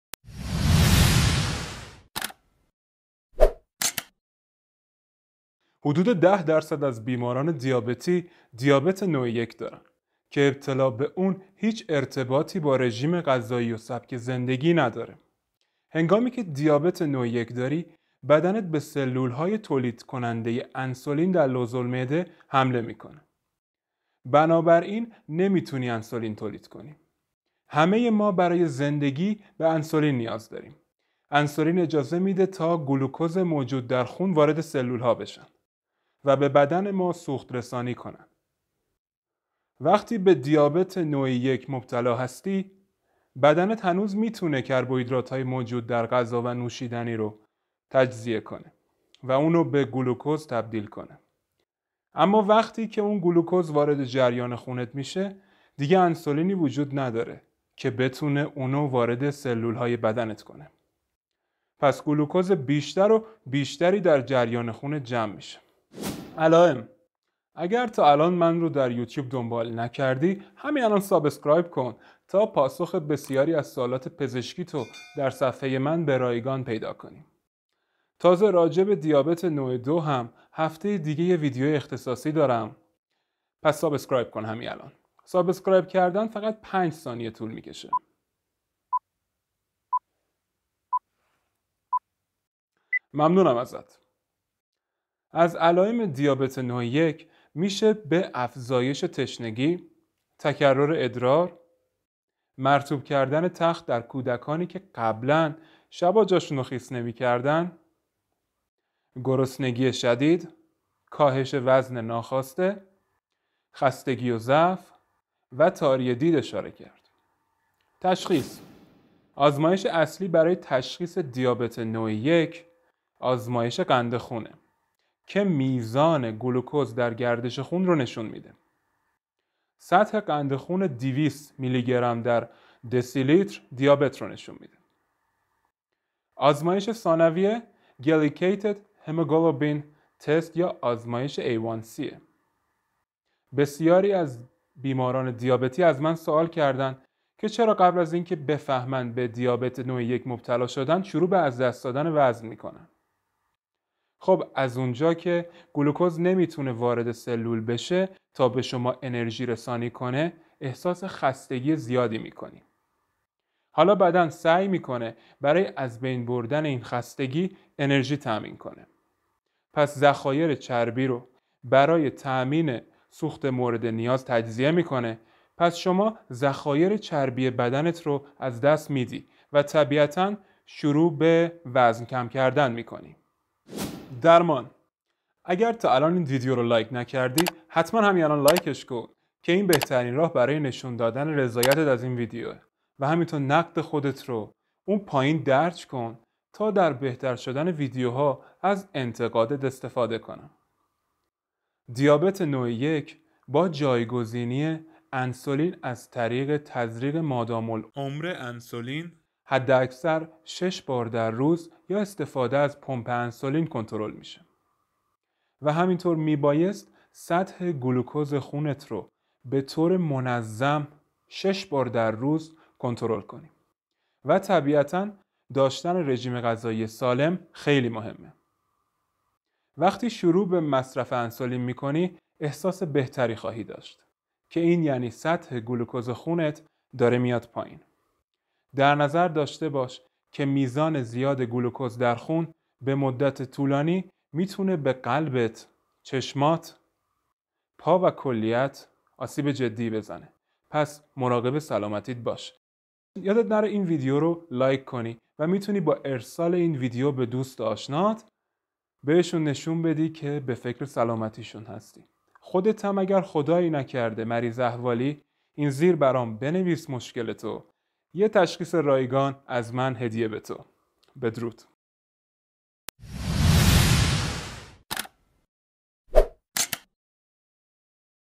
دیابت نوع ۱ - علت، علائم، تشخیص و درمان -Type 1 Diabetes - Causes,Symptoms, Treatment - EN RU Sub 4K
در این ویدیو خواهیم دید
دیابت نوع 1 چیست ؟
آیا دیابت نوع یک را جدی بگیریم ؟
علت ابتلا به دیابت نوع 1 چیست ؟
دیابت نوع یک چه علائمی دارد ؟
چگونه دیابت نوع یک را تشخیص دهیم ؟
چگونه دیابت نوع یک را درمان کنیم ؟
انسولین چند بار باید در روز تزریق شود ؟
اگر سوالی داشتید در بخش کامنت ها ذکر نمایید پاسخ میدم
In this video we will see :
What is type 1 diabetes?
Should we take type 1 diabetes seriously?
What causes type 1 diabetes?
What are the symptoms of type 1 diabetes?
How to diagnose type 1 diabetes?
How to treat type 1 diabetes?
How many times a day should insulin be injected?
if you have any questions ask bellow in the comment section
Attribution: In some of my videos I used free vectors & graphic elements